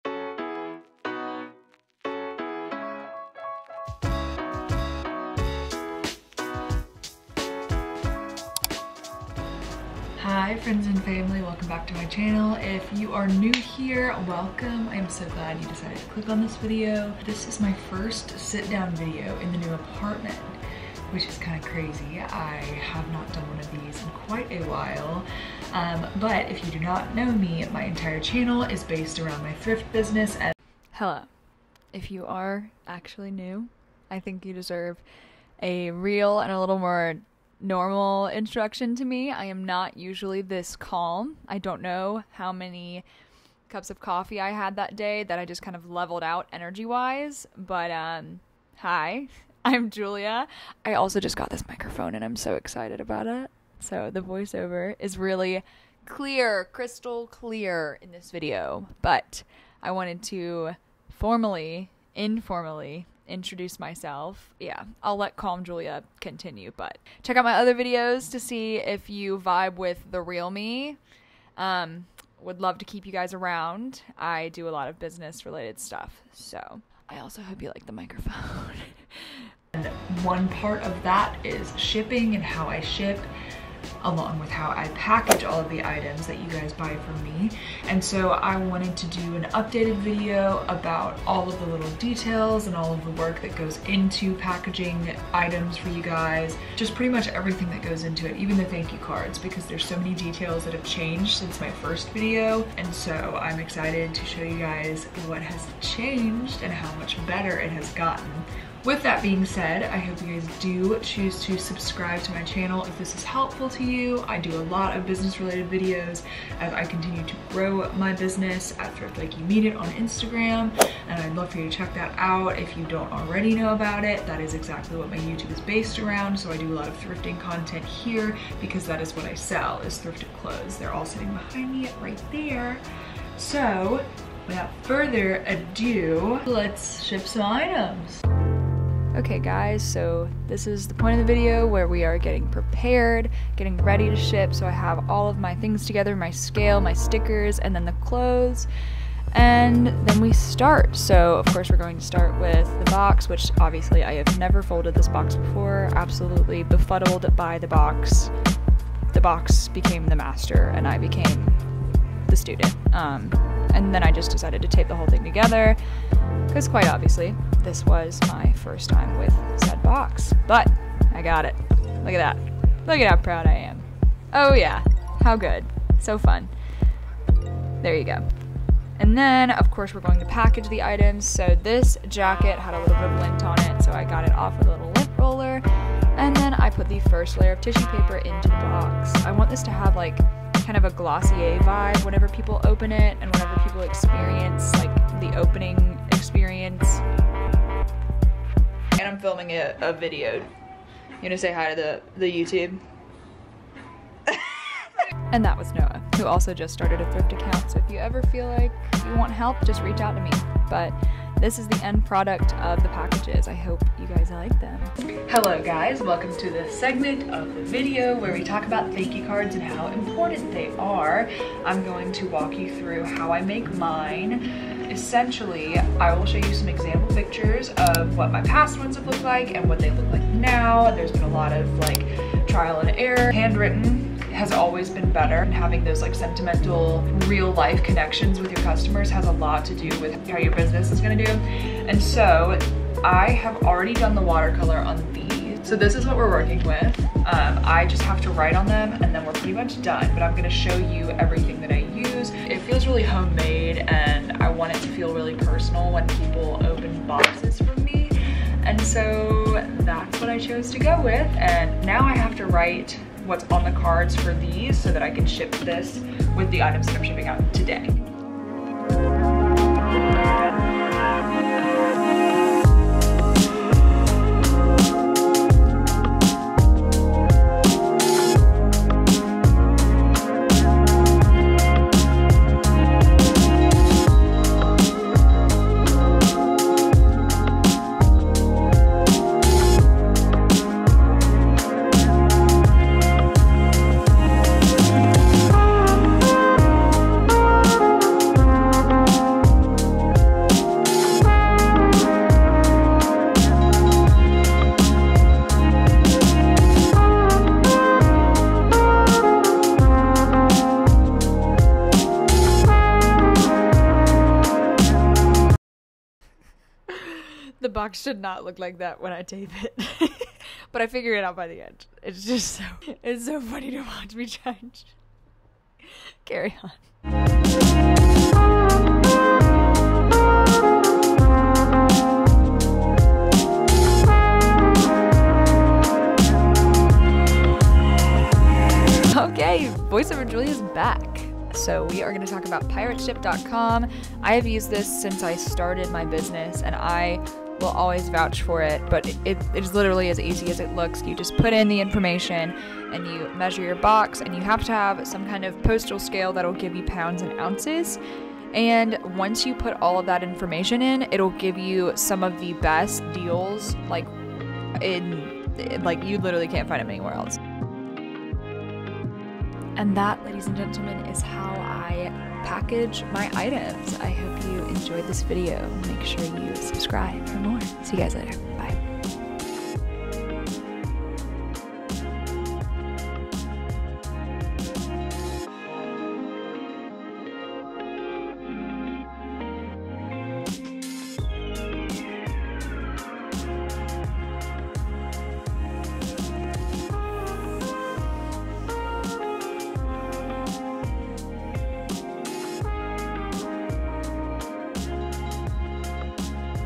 packaging and shipping orders for my small business!! ship cheap w USPS & DYMO LABELWRITER 450!
packaging and shipping orders for my small business!! ship cheap w USPS & more tips!
packaging and shipping orders for my small business!! ship cheap w USPS & DYMO LABEL PRINTER!

hi guys! today i decided to update you on what my shipping and packaging process looks like for my small business on instagram! I always like to provide i really personal and exciting experience for my friends who buy my items and i hope that shows in my packaging!

I really took a lot of. inspiration from the glossier makeup packaging, with the white box and the light and airy touches, but kept it really true to the brand!

White Boxes

Smiley Face Stickers

Tissue Paper is from the dollar store!

Similar Star Stamp

Similar Ink

Similar Paper

 Any watercolor set works!

DYMO LABELWRITER 450: